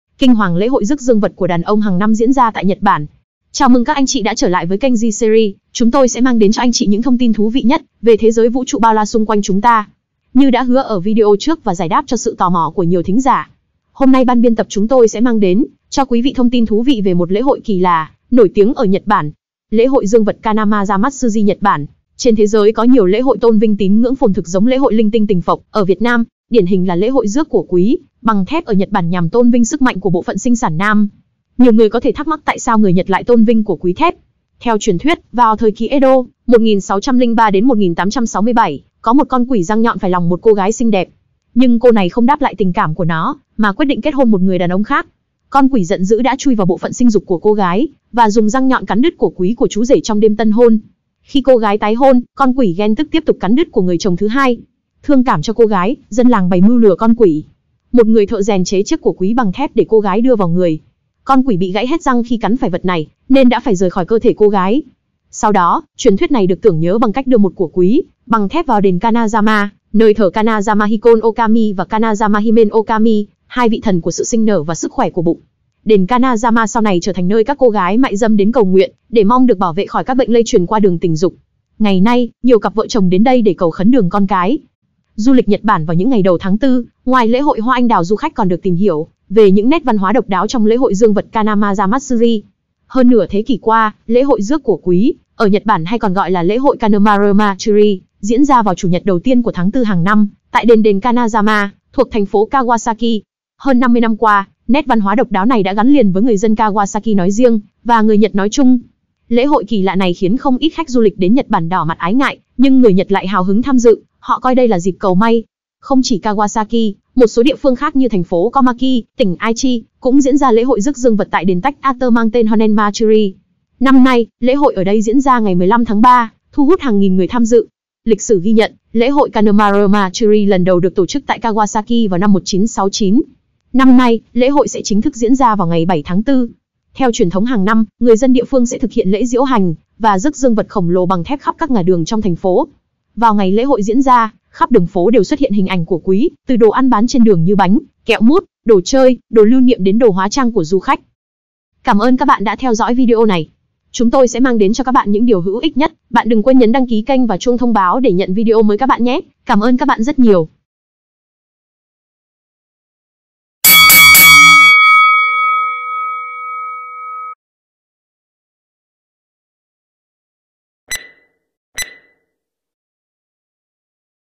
Kinh hoàng lễ hội rước dương vật quy mô tại Nhật Bản
Du lịch Nhật Bản vào những ngày đầu tháng 4, ngoài lễ hội hoa anh đào du khách còn được tìm hiểu về những nét văn hóa độc đáo trong lễ hội dương vật Kanamara Matsuri.
© Bản quyền thuộc về G-Series
© Copyright by G-Series ☞ Do not Reup
Admin: Admin